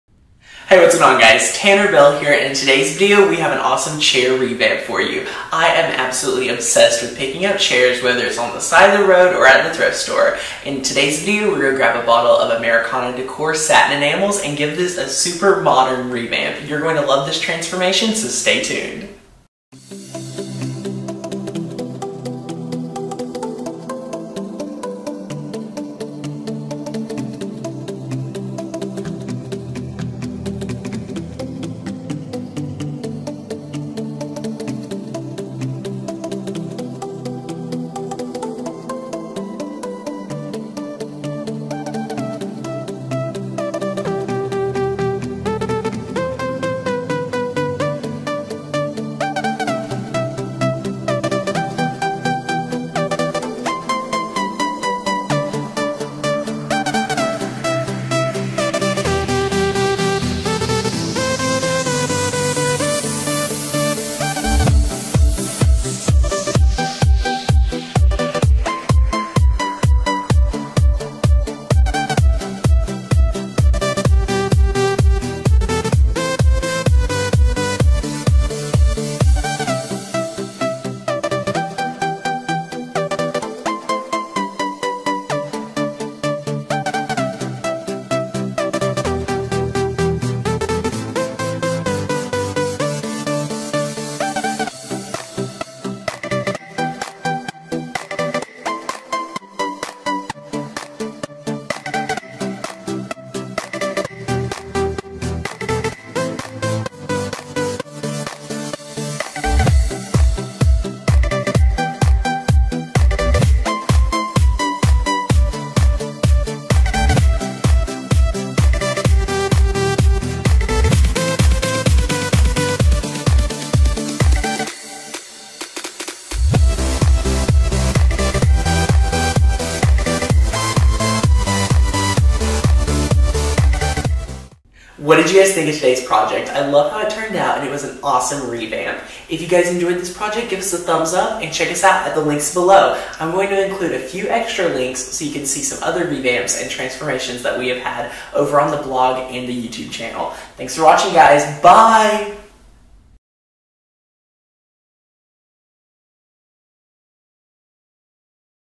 DIY Chair Revamp
I don't know about you, but I always find chairs that are in need of revamping, let me share with you how we used Americana Satin Enamels to totally transform this chair.

It's not as hard as you think to totally transform a thrift store chair with the help of Americana Decor Satin Enamels.